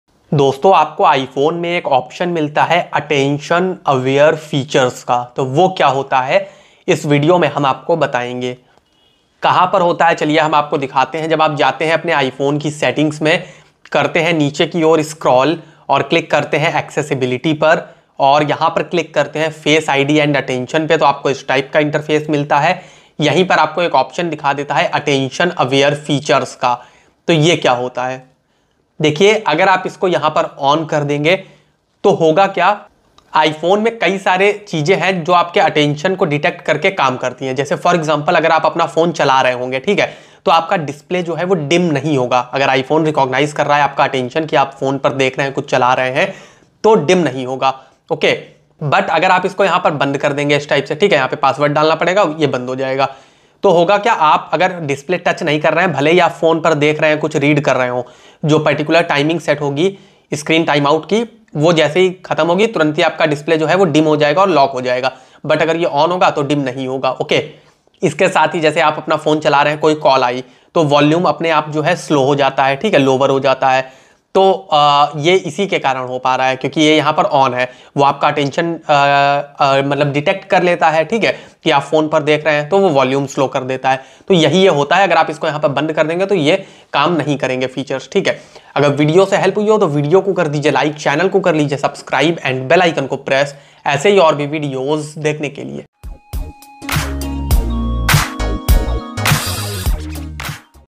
iPhone Me Attention Aware Features Kya Hota Hai | What Is Attention Aware Features In iPhone | Hindi
1) iphone me attention aware features kya hota hai
2) what is attention aware features in iphone
3) iphone attention aware features meaning in hindi